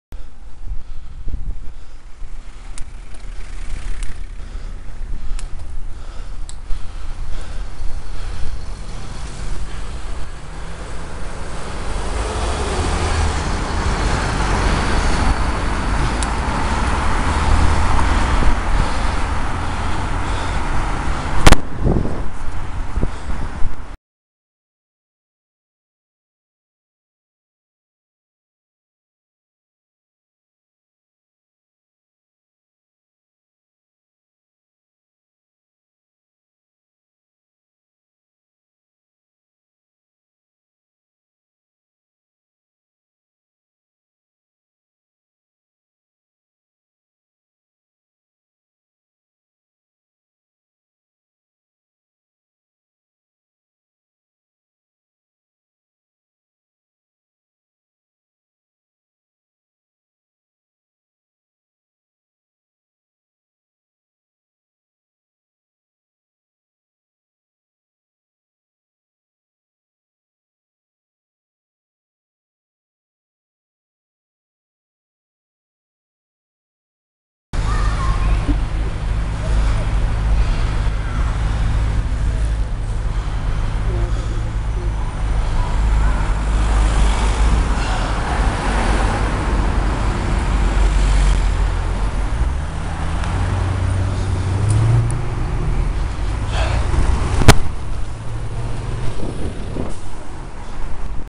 Navigation Bridge
The other day I was out on a ride and found this little gem. Using this means I get to avoid Foss Islands road :D

I'm not sure it's the right name, but it seems fitting as it's off Navigation Road